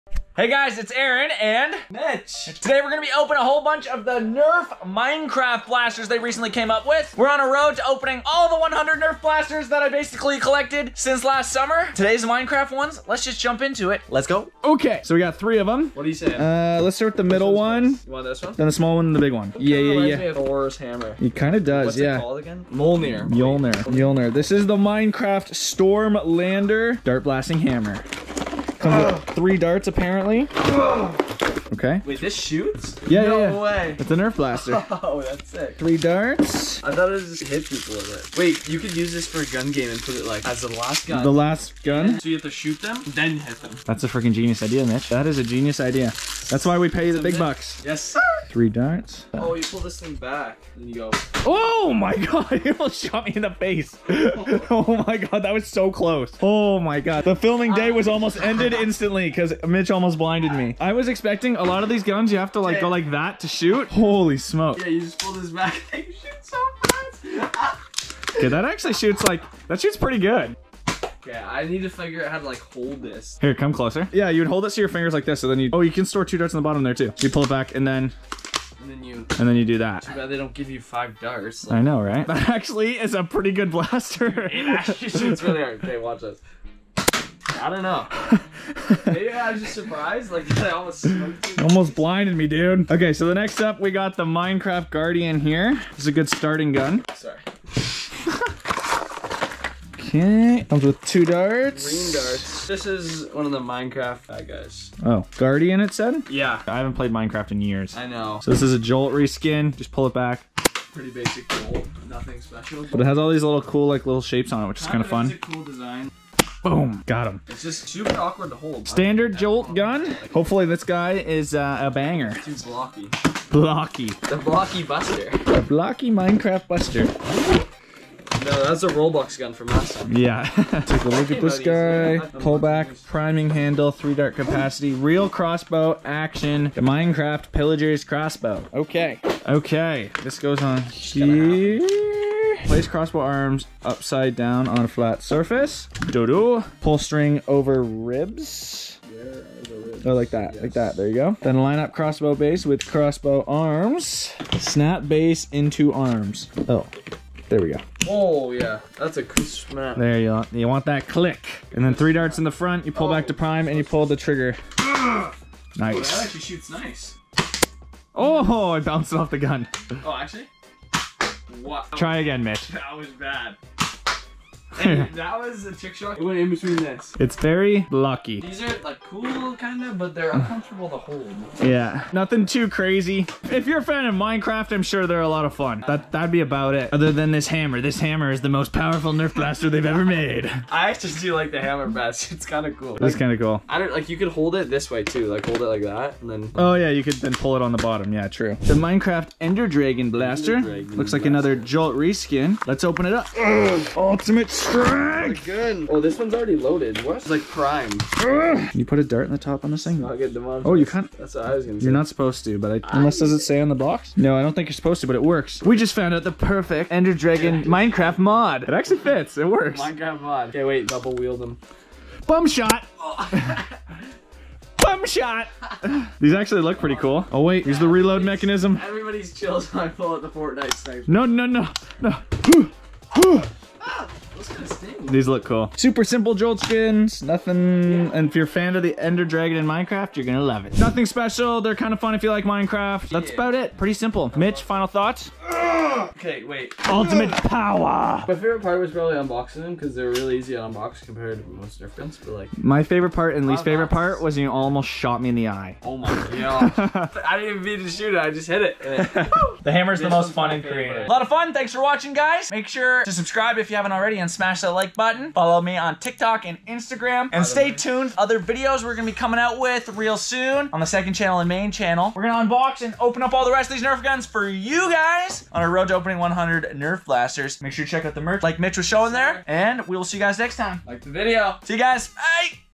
NERF MINECRAFT GUNS!
Unboxing and  review of the latest Nerf Minecraft Blasters! The Pillager's Crossbow, Stormlander, Microshot Ender Dragon, and Guardian Mini Blaster!